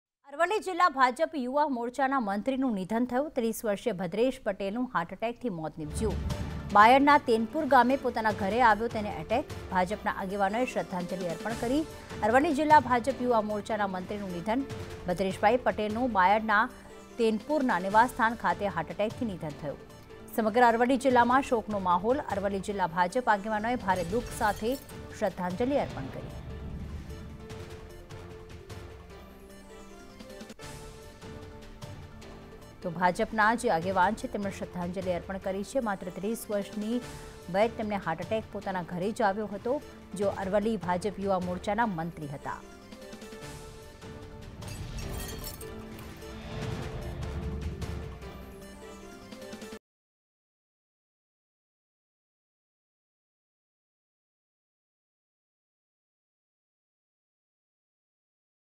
Aravalli News | અરવલ્લી જિલ્લા ભાજપ યુવા મોરચાના મંત્રીનું નિધન | Aravalli District | BJP
અરવલ્લી જિલ્લા ભાજપ યુવા મોરચાના મંત્રીનું નિધન
30 વર્ષીય ભદ્રેશ પટેલનું હાર્ટ એટેકથી મોત
બાયડના તેનપુર ગામે પોતાના ઘરે આવ્યો એટેક
ભાજપના આગેવાનોએ શ્રદ્ધાંજલિ અર્પણ કરી